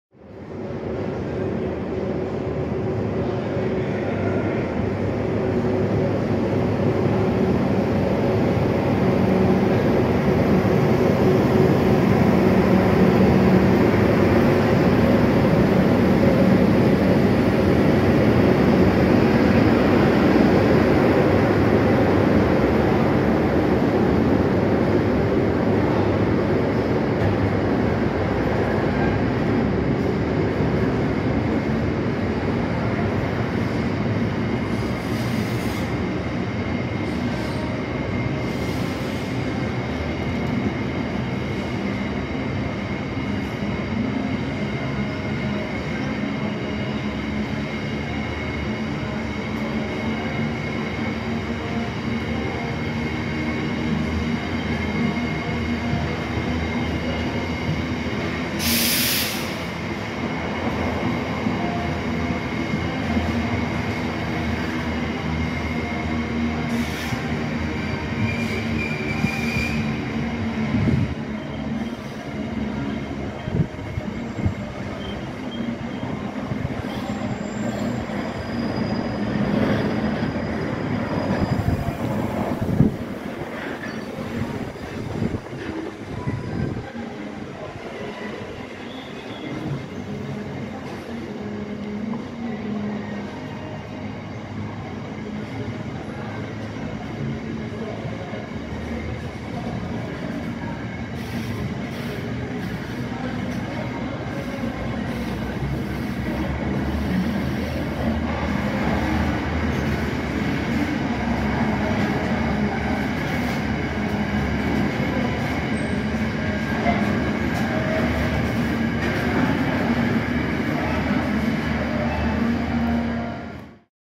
Class 90 double head freightliner train trough Manchester, Great Britain
30th of June 2023